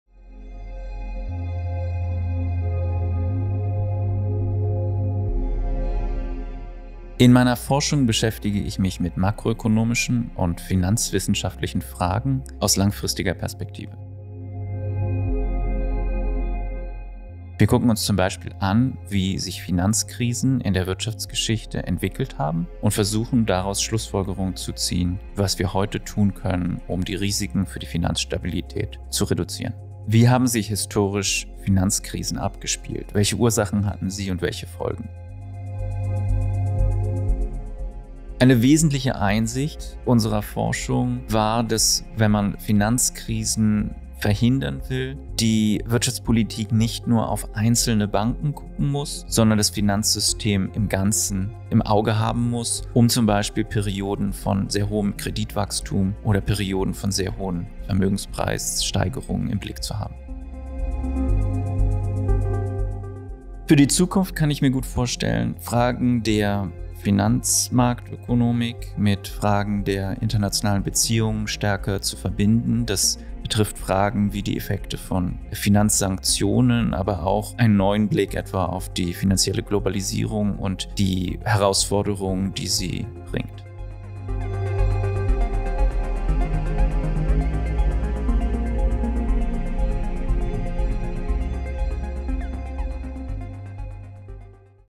Gottfried Wilhelm Leibniz-Preisträger 2022 Moritz Schularick im Porträt
Filmporträt anlässlich des Gottfried Wilhelm Leibniz-Preises der Deutschen Forschungsgemeinschaft (DFG) 2022

Fotos: ECONtribute: Markets & Public Policy